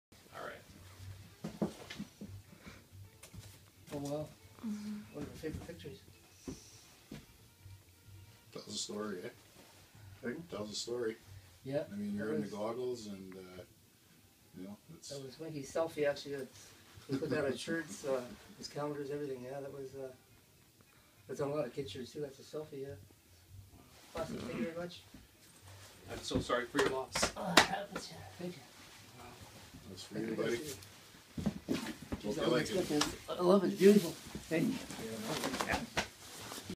Winky's Last Ride unveiled
On Thanksgiving Sunday, Sandy MacDonald was brought to Birdsong Studio by his friends Kelly and Koop. He didn't know why. This was the moment he saw Winky's Last Ride for the first time.